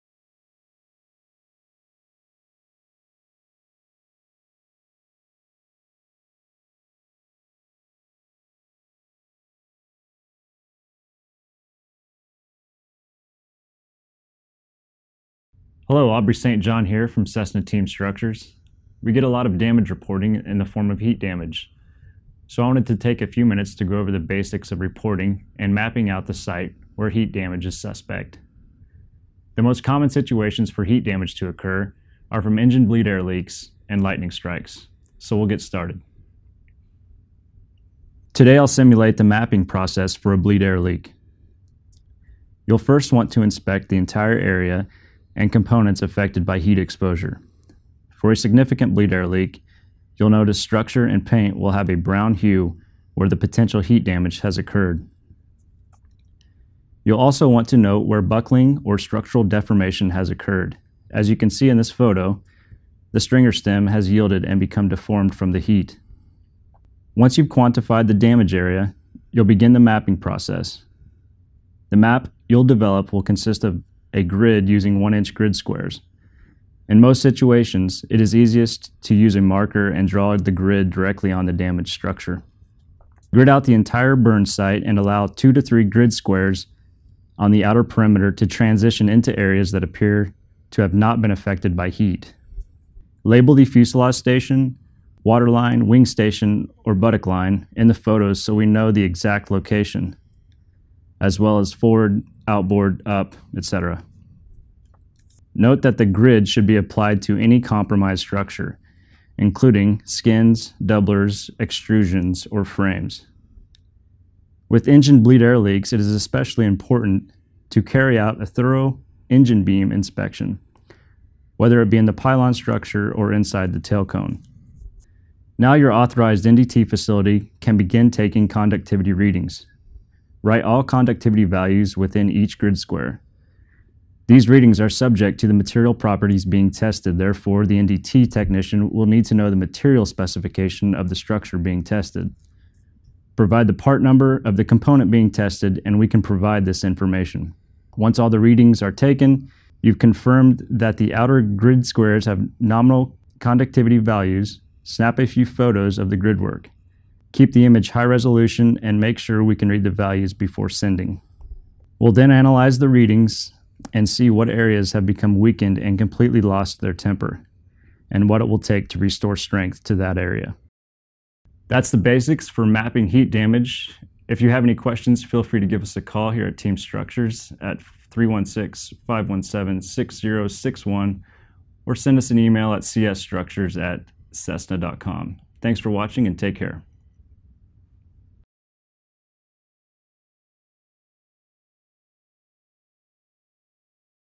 Heat Damage Mapping
Aubrey St. John demonstrates how to map heat damage when reporting damage to Cessna Team Structures.